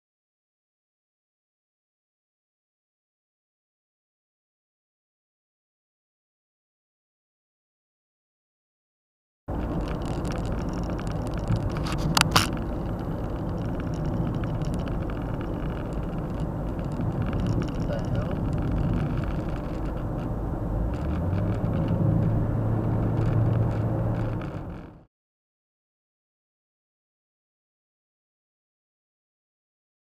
Harvester On The Highway [HD]
(16:9, 720P)
Dashcam footage of a cane harvester travelling alongside the highway.

VIDEO CODE: PFGC038